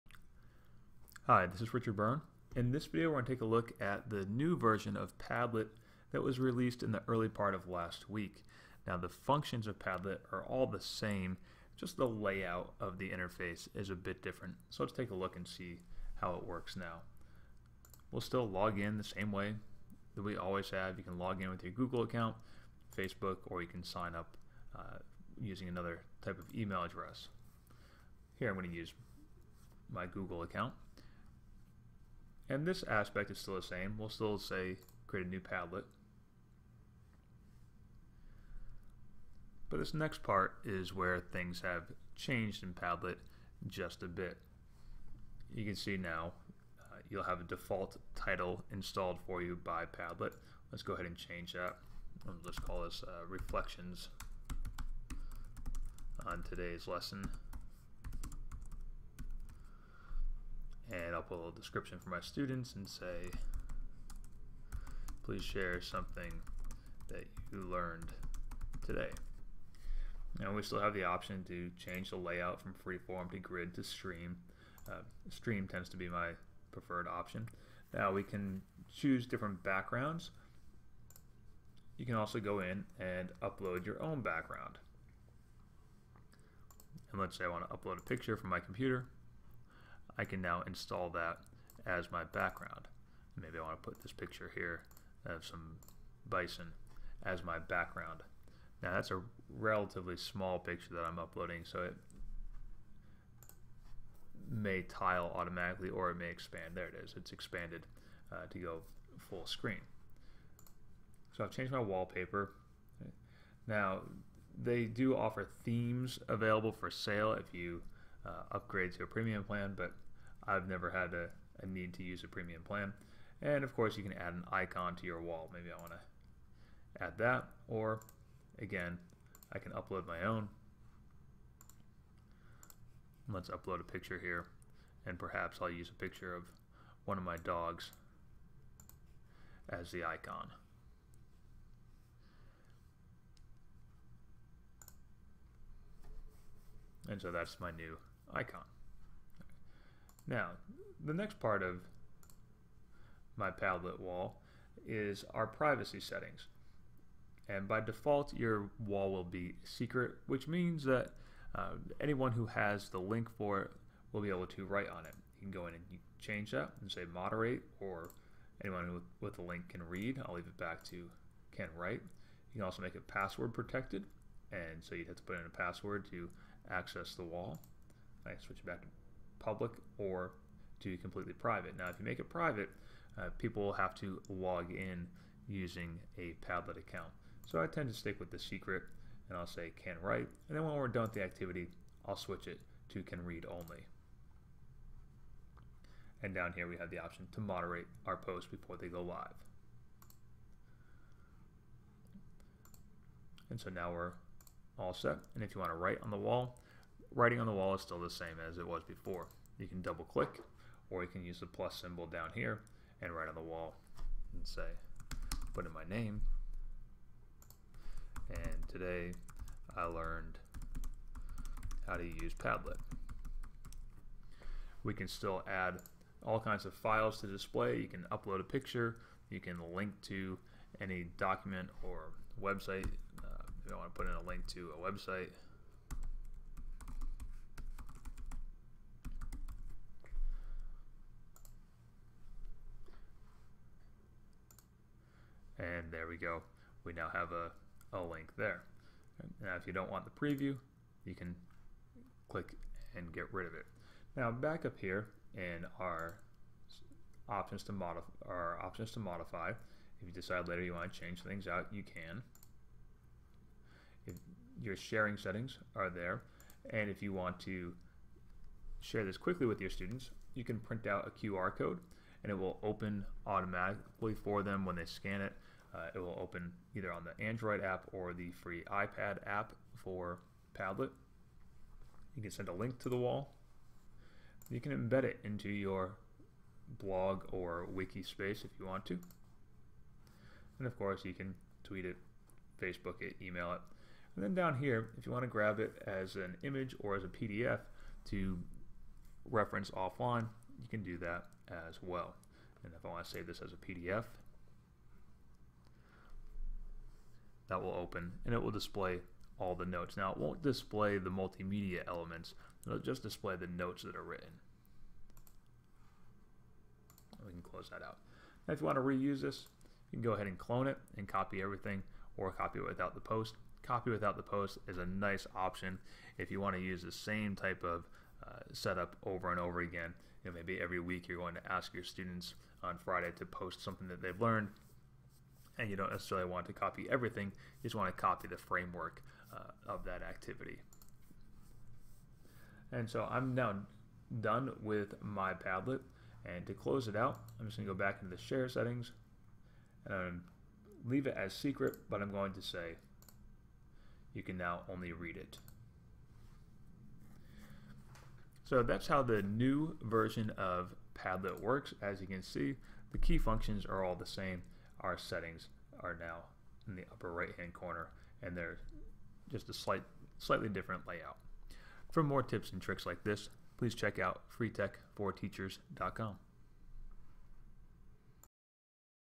How to Use the New Version of Padlet
How to use the new version of Padlet.com to create custom online corkboards.